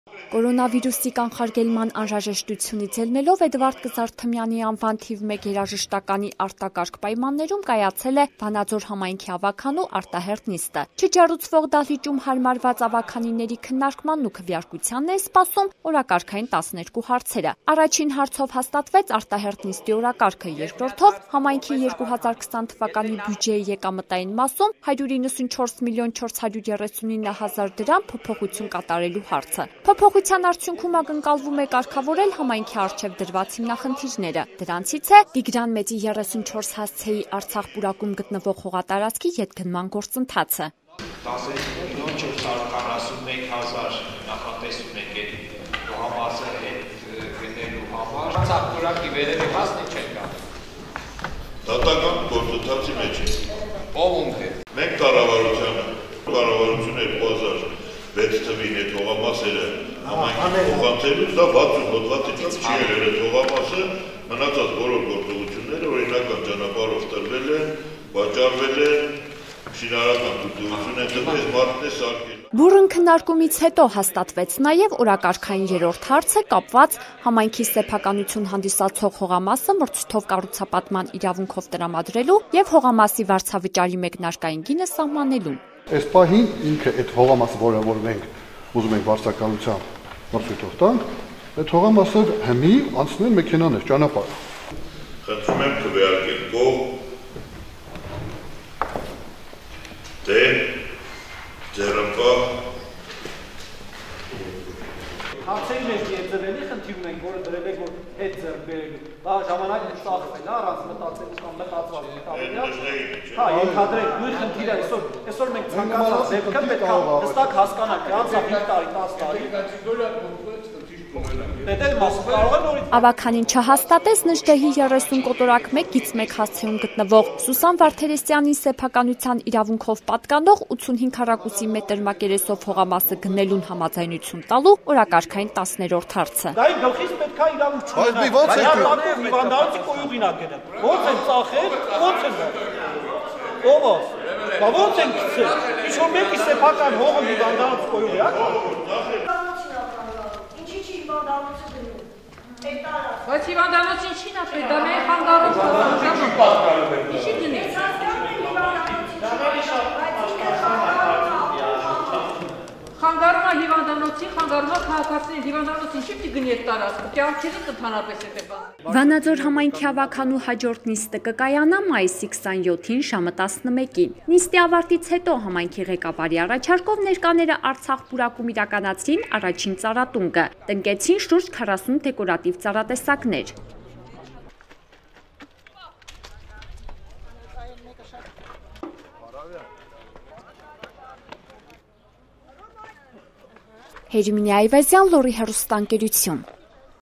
«Արտակարգ» պայմաններում կայացած Վանաձորի ավագանու արտահերթ նիստը՝ վիճահարույց հարցերով
Կորոնավիրուսի կանխարգելման անհրաժեշտությունից ելնելով Էդ. Կզարթմյանի անվան թիվ 1 երաժշտականի արտակարգ պայմաններում կայացել է Վանաձոր համայնքի ավագանու արտահերթ նիստը: Չջեռուցվող դահլիճում հարմարված ավագանիների քննարկմանն ու քվեարկությանն էին սպասում օրակարգային 12 հարցերը: Առաջին հարցով հաստատվեց արտահերթ նիստի օրակարգը, 2-րդ ով  համայնքի 2020թ.-ի բյուջեյի եկամտային մասում 194 մլն 439 հազար դրամ փոփոխություն կատարելու հարցը:  Փոփոխության արդյունքում ակնկալվում է կարգավորել համայնքի առջև դրված հիմնախնդիրները, դրանցից է   Տիգրան Մեծի   34 հասցեի( Արցախ Պուրակում) գտնվող հողատարածք ետգնման գործընթացը: Վանաձոր համայնքի ավագանու հաջորդ նիստը կկայանա մայիսի 27-ին, ժամը 11:00-ին: Նիստի ավարտից հետո համայնքի ղեկավարի  առաջարկով ներկաները Արցախ Պուրակում իրականացրին առաջին ծառատունկը, տնկեցին շուրջ 40 դեկորատիվ ծառատեսակներ: